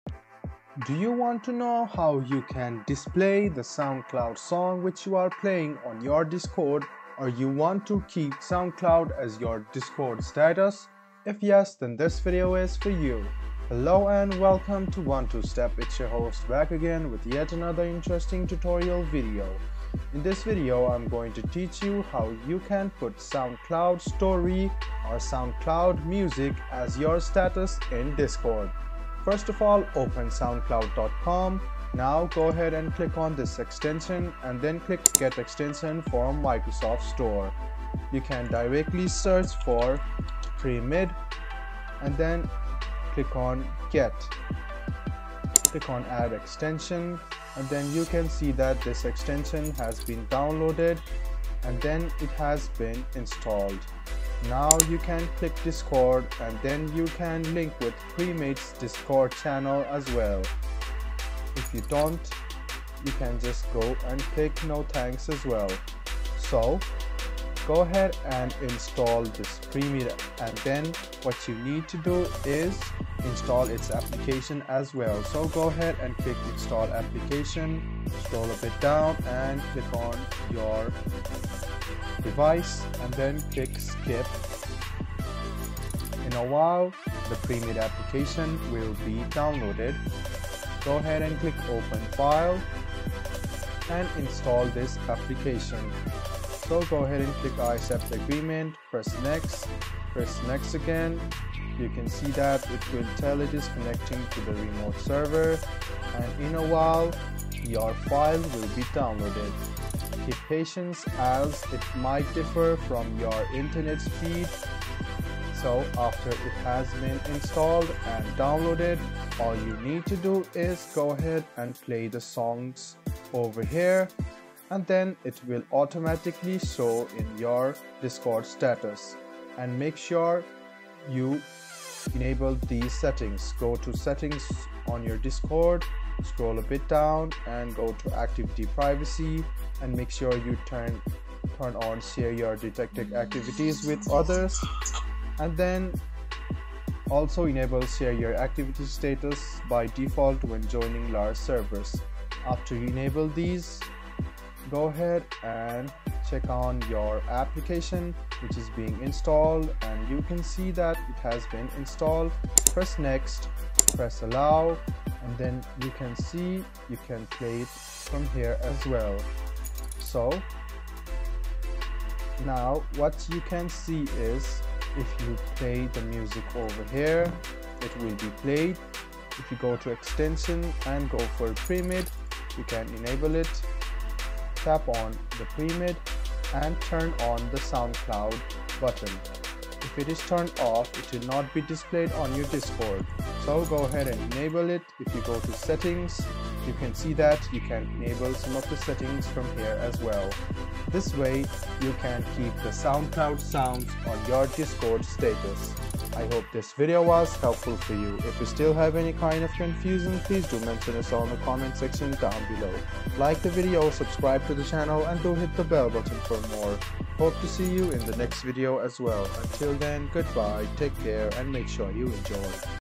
How to Display SoundCloud Song You’re Playing on Discord (2024) | SoundCloud Tutorial
Discover how to share the SoundCloud song you're currently playing directly on your Discord status. This quick tutorial will walk you through setting up Discord to automatically display your SoundCloud tracks to friends.